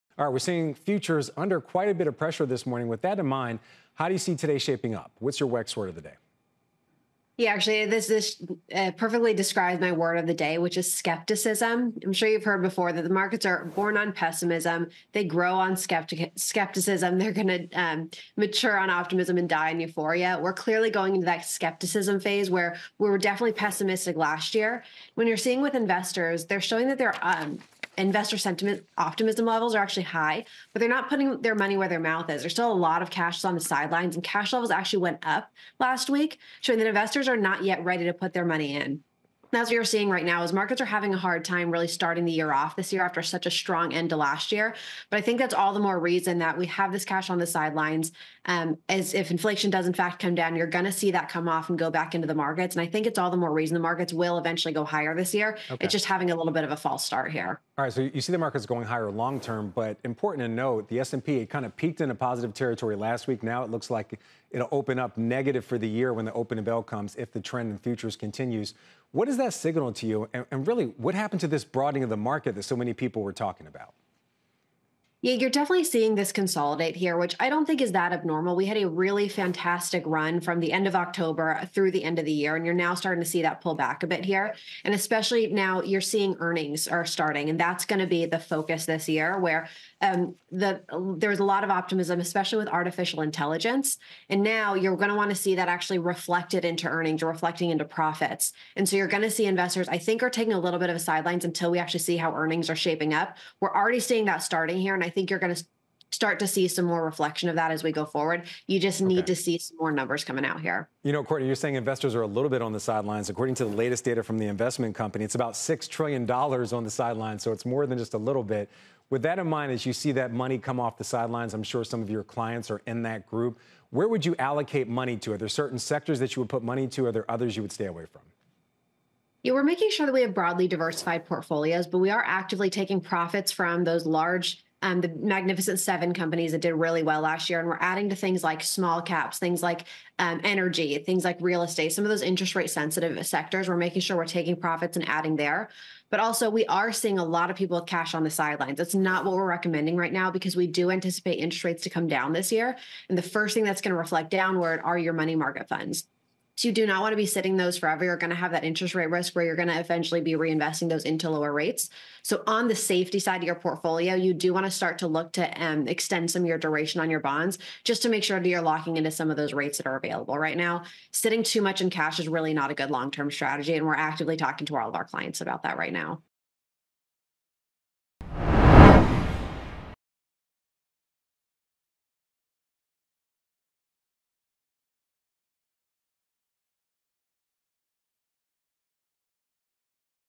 Investors aren't putting their money where their mouth is, says Courtney Garcia
Courtney Garcia, Senior Wealth Advisor at Payne Capital Management, discusses the market action this week.